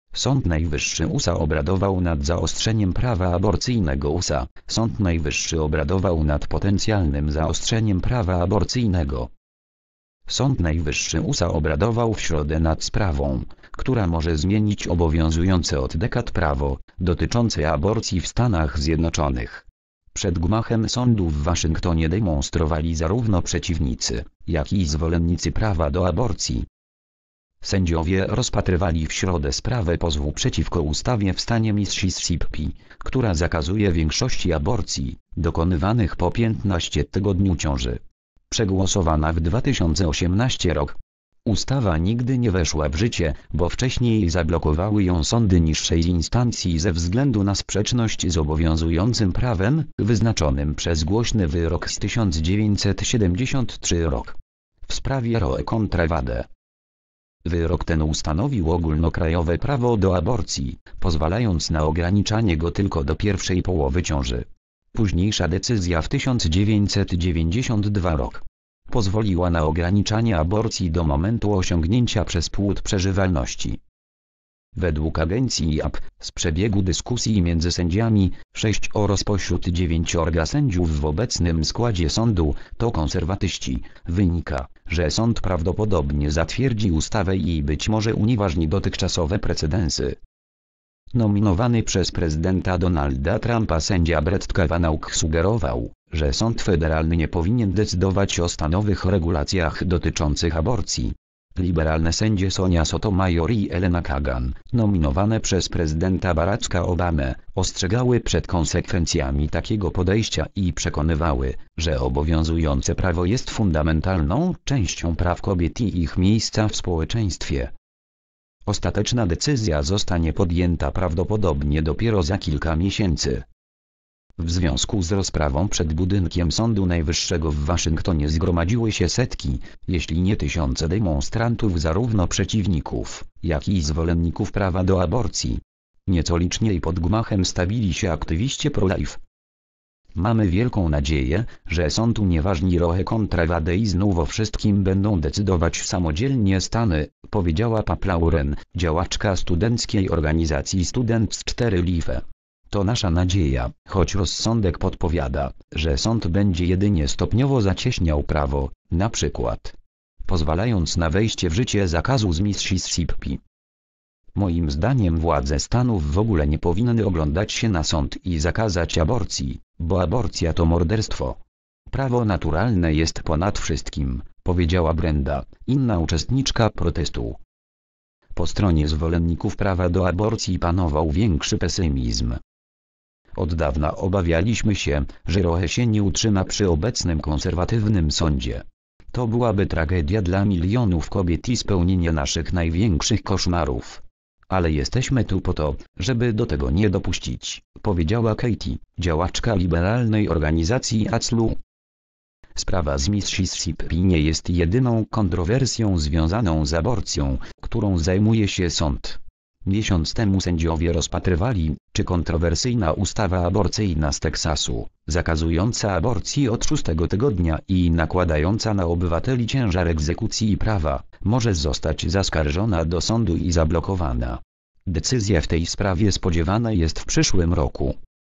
Sąd Najwyższy USA obradował nad zaostrzeniem prawa aborcyjnego.
USA: Sąd Najwyższy obradował nad potencjalnym zaostrzeniem prawa aborcyjnego.

  (fot. PAP/EPA/SAMUEL CORUM)